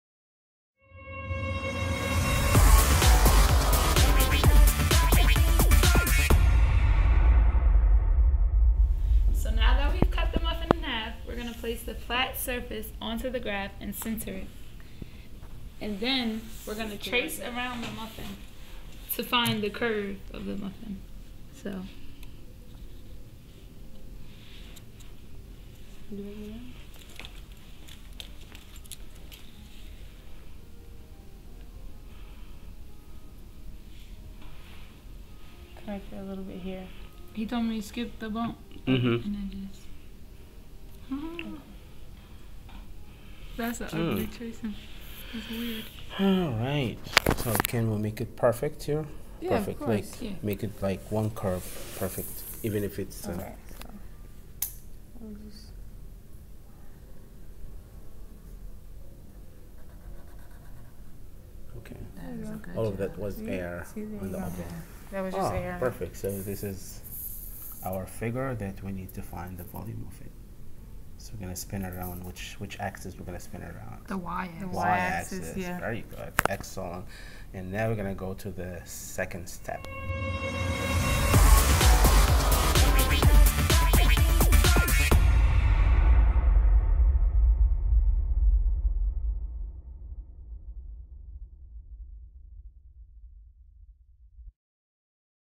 Applying Integration on Muffin - part 3
Students teach the application of integration on a muffin